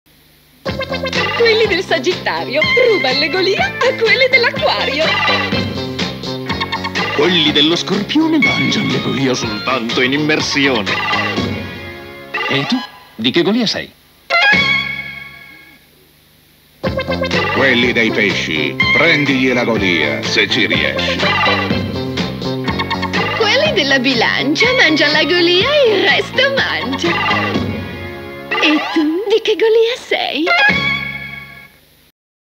2 Spot - GOLIA Caramelle (ASTROGOLIA) - 1984 *RaRo*
Pubblicità anni 80 delle caramelle golia. Serie di spot ASTROGOLIA.